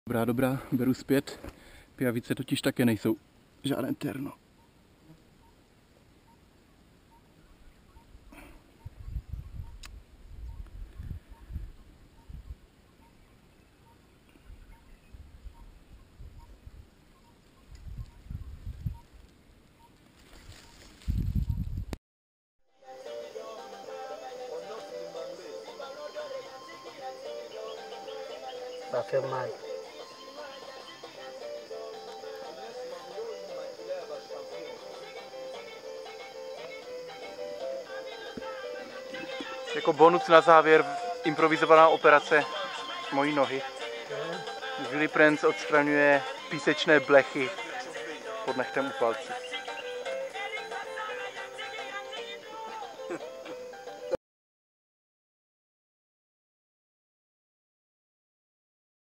Chigoe fleas (Tunga penetrans), liandi mabe na loboko ya mondele | Save-elephants.org
Ať už je Dzebe sebekrásnější ráj, pořád leží v srdci drsné rovníkové Afriky se spoustou bezobratlé žoužele, která se vám ráda zavrtá pod kůži -- a to doslova. Následující video obsahuje záběry, ze kterých se nemusí dělat každému dobře. Pijavice a písečné blechy (Tunga penetrans) nejsou sice nebezpečné a dokonce ani bolestivé, ale je lepší je nemít... (více
Despite the beauty all around, Dzebe is in the middle of the tropical rainforest, which sometimes can be mischievous. There is plenty of invertebrates which really like being close to you -- or inside you. Literally... Some viewers will not feel comfortable watching these footages. Leeches and chigoe fleas (Tunga penetrans) are not painful and dangerous (in small amounts!), but it is definitely better to avoid them, anyway. (more